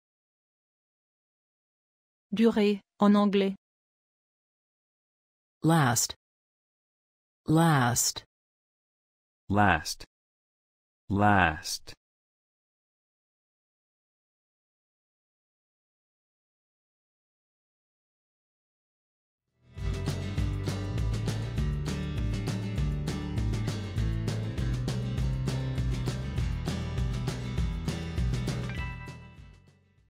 Comment dire durer en anglais
Comment dire correctement durer en anglais? Ecoute avec nous.
Quelle est la traduction correcte du mot durer du français vers l'anglais? Parlez comme les Américains. Aussi, comment écrire correctement le mot last?